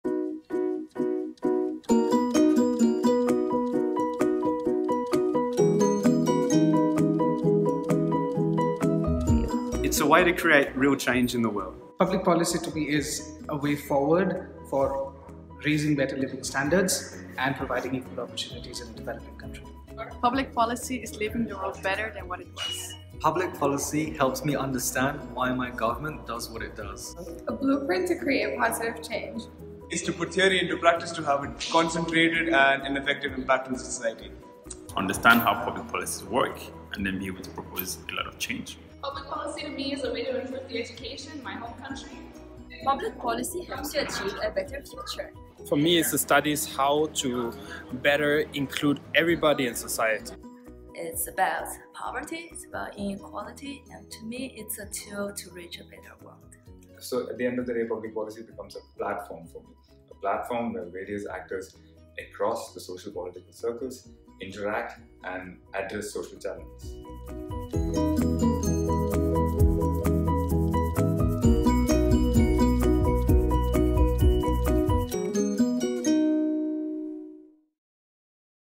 What is Public Policy, actually?
What is public policy? And why is it important?

We asked our Master's students in Public Policy and Human Development to tell us what they thought.

This video clip was conceived and produced by the DEMOS Study Association at United Nations University-MERIT and Maastricht University, as a teaser for our Master's Open Day on Saturday 17 November.

For more information about our one-year, double-degree Master's programme, please